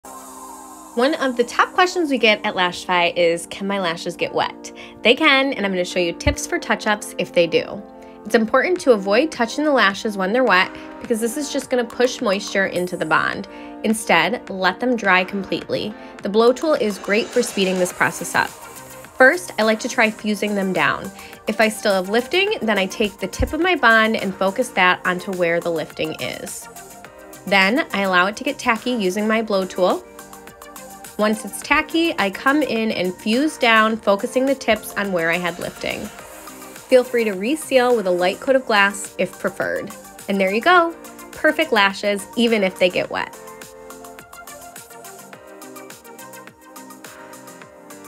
How to Touch Up Wet Lashes
How to touch up your Lashify Gossamer® lashes if they become wet